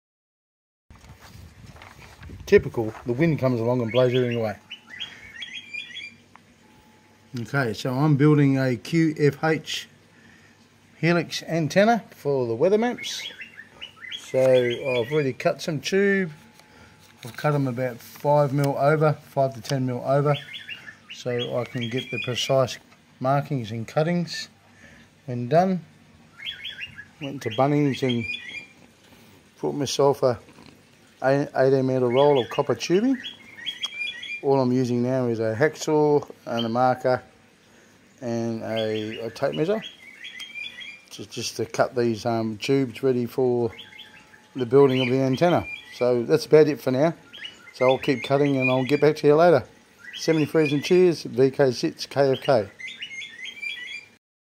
Building a QFH Antenna
Building a QFH Antenna for downloading weather maps